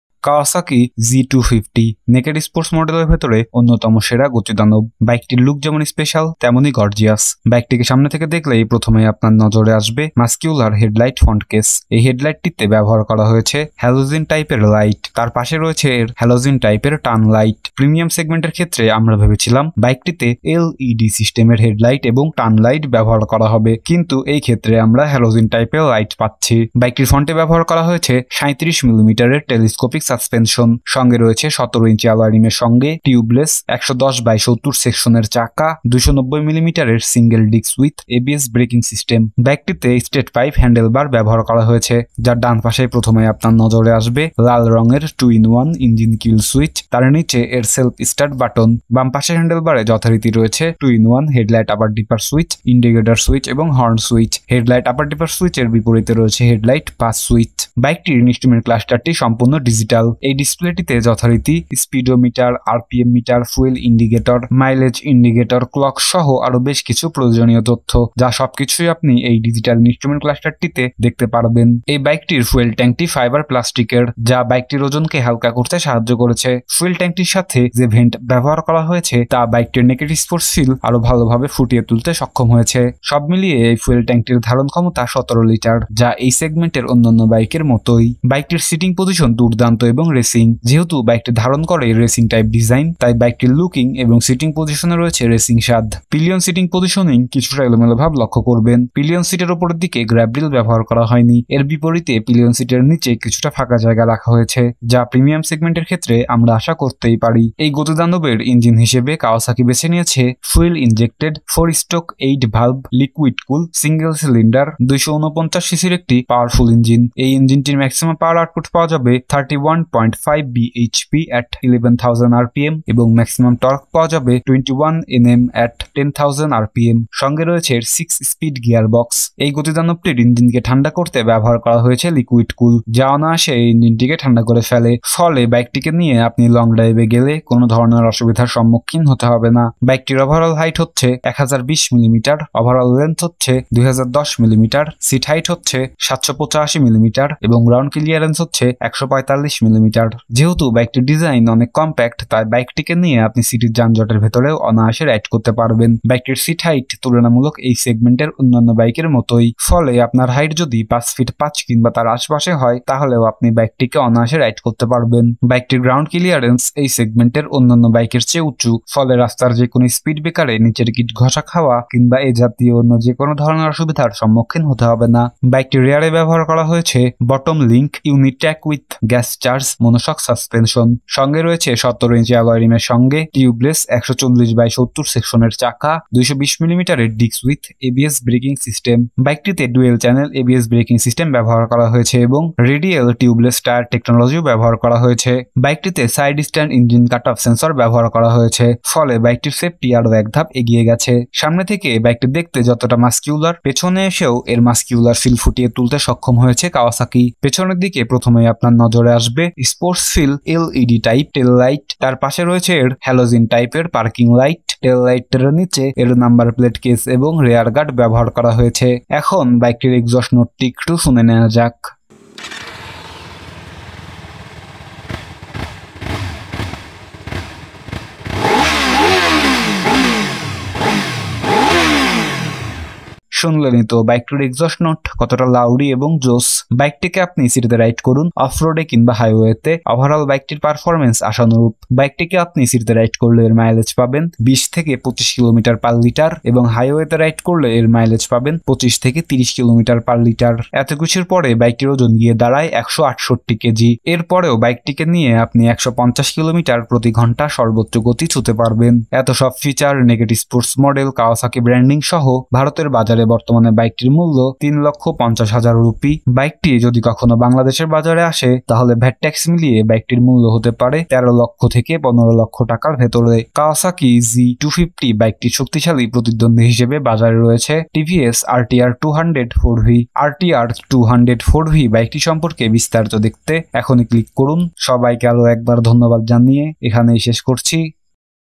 Kawasaki Z250 : Full Review in Bangla !!
The Kawasaki Z250 is one of the best names inside the Naked Sports model. The look of the bike is as special as it looks gorgeous from the front or from the back.

When you see the bike from the front, the first thing that comes to your notice is the non-reflector, Weiser , Used in the font of the bike , Telescopic suspension, The bike uses a state pipe handle bar, The bike uses a complete digital instrument cluster, The fuel tank is made of fiber plastic which has helped to lighten the weight of the bike, the grille used with the fuel tank has been able to better enhance the naked sports feel of the bike.

The bike installment team position is great and since racing contains this racing type design so the bike has racing in the looking and setting position.
The bike uses dual channel ABS braking system, and radial tubeless tire technology.
I am here to thank everyone once again.

Quries Solved :

Specification: Kawasaki Z250
1. Details
CC Category  250 cc

2. Engine & Transmission
Engine Type Fuel Injection 4-stroke 8-valve ,Liquid Cooled 249 cc
Max Power 31.5 bhp @ 11,000 rpm
Max Torque 21 Nm @ 10,000 rpm
Gears  6

Clutch   Wet, Multi Plate Clutch
Engine Cooling  Liquid Cooled
Fuel Supply  Fuel Injection
Starting Method  Electric Starter

3. Dimensions

4. Brakes, Wheels & Suspensions
Braking   Dual Channel ABS
Front Brake  290 mm Disc
Rear Brake  220 mm Disc
Front Suspension Telescopic with Anti-friction Bush
Rear Suspension  Bottom-Link Uni-Trak with gas-charged shock
Front Tyre  110/70-17

5. Top Speed and Mileage
Mileage (Average) 20-30 km (Approx)
Top Speed  150 Kmph (Approx)

6. Electricals
Battery  12V MF
Headlight Halogen
Tail Light LED
Indicators Halogen

7. Features
Engine Kill Switch Yes
Handle Type  3-Part Handle Bar
Speedometer  Digital
RPM Meter  Digital
Odometer  Digital
Seat type  Split